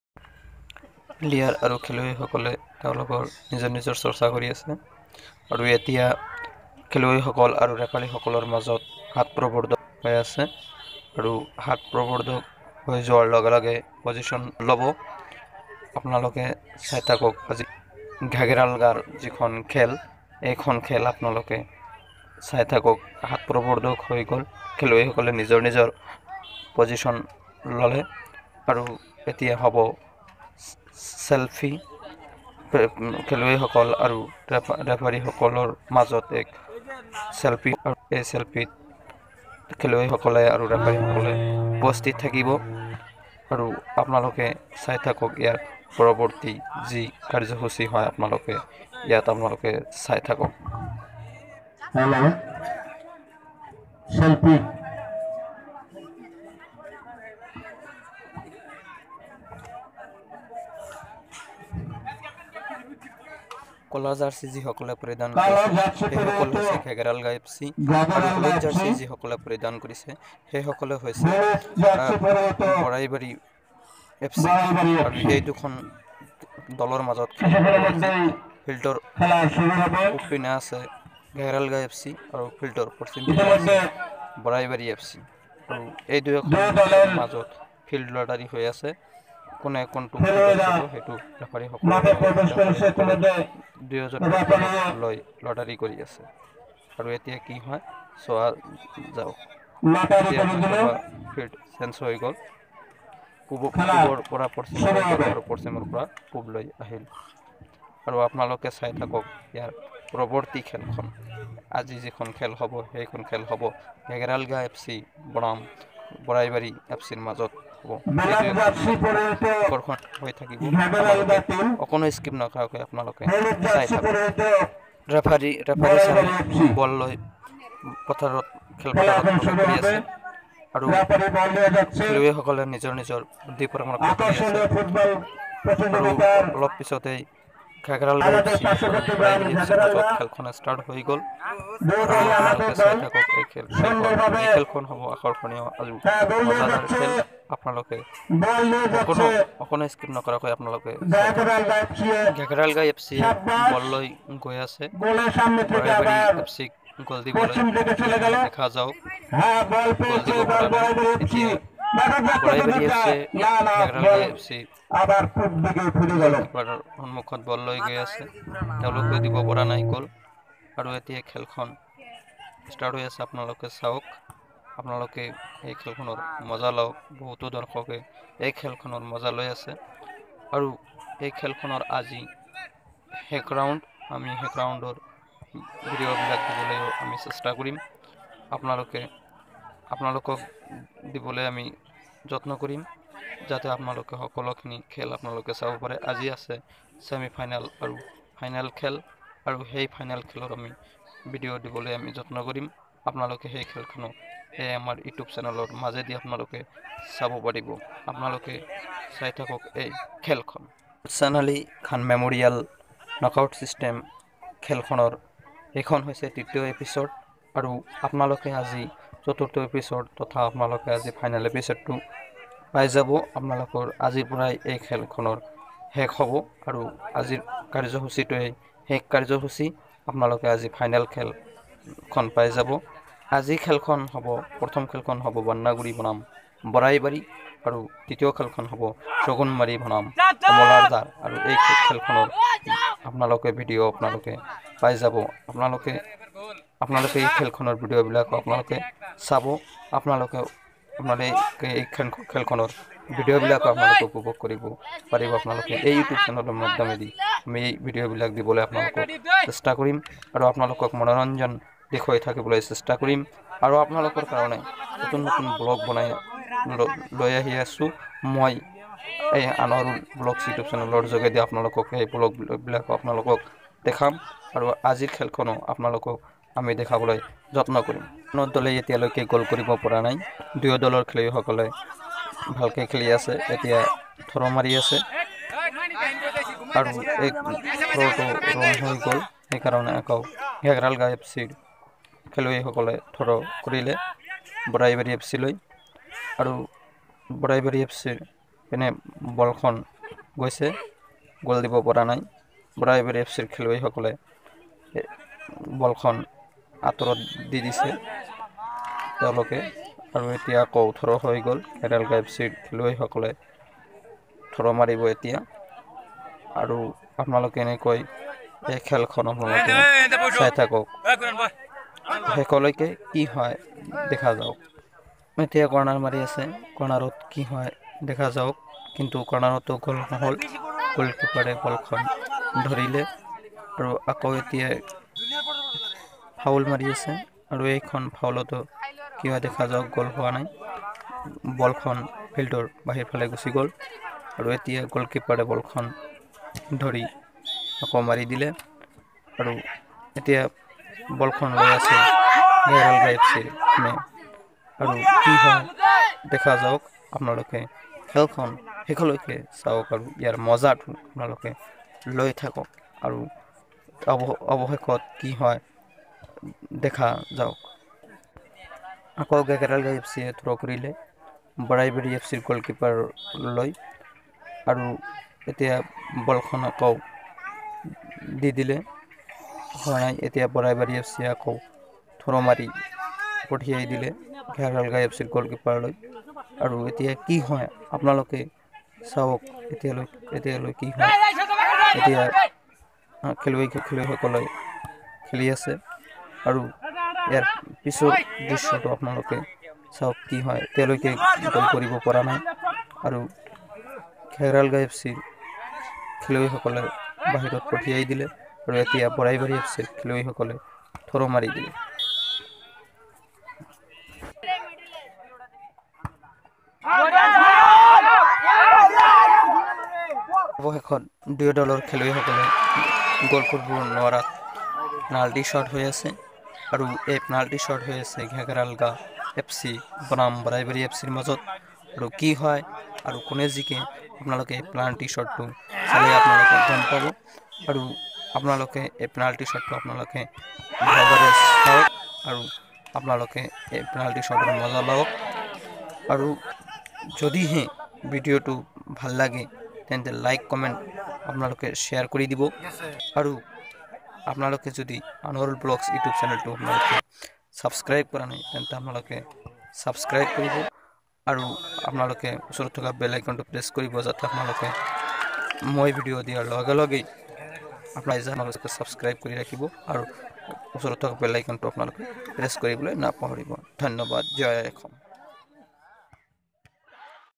Football Tournament Barai bari BYPAS V/S Ghager Alga FC
This is football tournament between Baraibari FC V/S Ghager Alga FC. Barai bari team is win this football match.